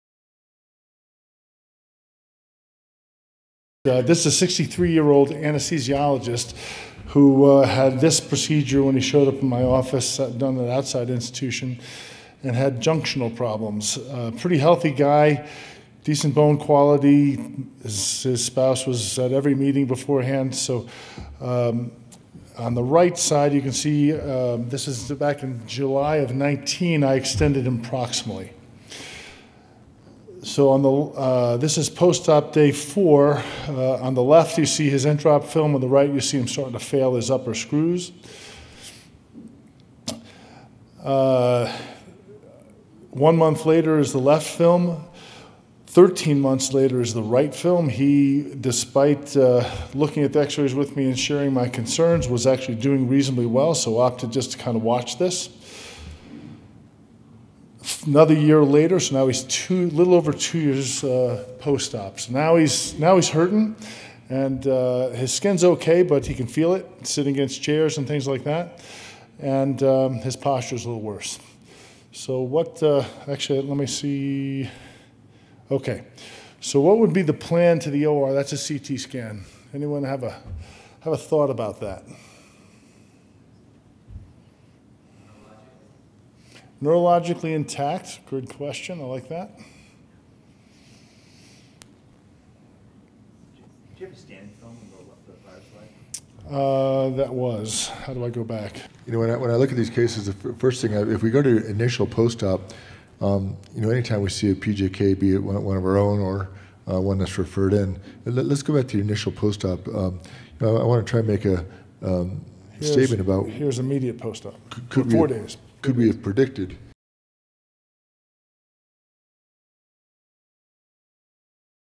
Selby 2023 TL Spine - Cliff Tribus, MD
New expert video by Cliff Tribus, MD of UW Health at the 2023 Selby Spine Conference.